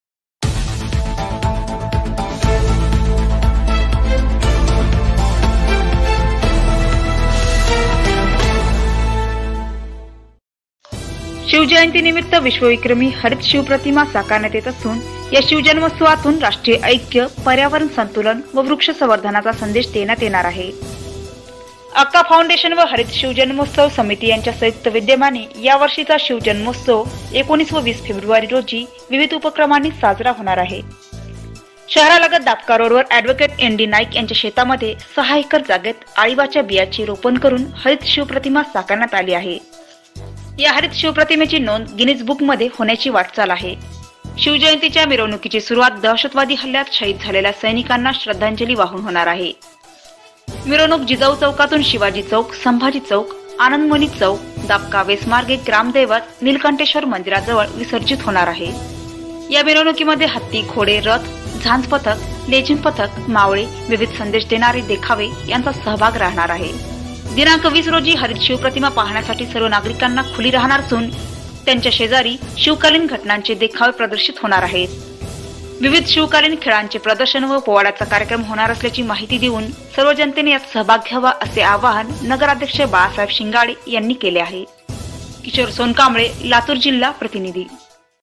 पाहा लातुरात शिवाजी महाराजांची विश्व विक्रमी हरित शिव प्रतिमा...
विश्व विक्रमी हरित शिव प्रतिमा..

किशोर सोनकांबळे
लातूर जिल्हा प्रतिनिधी

    शिवजयंतीनिमित्त विश्वविक्रमी हरित शिवप्रतिमा साकारण्यात येत असून या शिवजन्मोत्सवातून राष्ट्रीय ऐक्य, पर्यावरण संतुलन व वृक्ष संवर्धनाचा संदेश देण्यात येणार आहे.
       अक्का फाऊंडेशन व हरित शिवजन्मोत्सव समिती यांच्या संयुक्त विद्यमाने यावर्षीचा शिवजन्मोत्सव 19 व 20 फेब्रुवारी रोजी विविध उपक्रमांनी साजरा होणार आहे.
      शहरालगत दापका रोडवर अ‍ॅड.एन.डी. नाईक यांच्या शेतामध्ये सहा एकर जागेत आळीवाच्या बियाची रोपण करून हरीत शिवप्रतिमा साकारण्यात आली आहे. या हरित शिवप्रतिमेची नोंद गिनीज  बुकमध्ये नोंद होण्याची वाटचाल आहे.
 शिवजयंतीच्या  मिरवणुकीची सुरुवात दहशवादी हल्ल्यात शहीद झालेल्या सैनिकांना श्रद्धांजली वाहून होणार आहे. मिरवणूक जिजाऊ चौकातून शिवाजी चौक, संभाजी चौक, आनंद मुनी चौक, दापका वेस मार्गे ग्रामदैवत नीलकंठेश्वर मंदिराजवळ विसर्जित होणार आहे. या मिरवणुकीत हत्ती, घोडे, रथ, झांज पथक, लेझीम पथक, मावळे, विविध संदेश देणारे देखावे यांचा सहभाग राहणार आहे. 20 रोजी हरित शिवप्रतिमा पाहण्यासाठी सर्व नागरिकांना खुली राहणार असून त्याच्या शेजारी शिवकालीन घटनांचे देखावे प्रदर्शित होणार आहेत.” विविध शिवकालीन खेळांचे प्रदर्शन व पोवाड्याचा कार्यक्रम होणार असल्याची माहिती देऊन सर्व जनतेने यात सहभाग घ्यावा, असे आवाहन नगराध्यक्ष बाळासाहेब शिंगाडे यांनी केले आहे.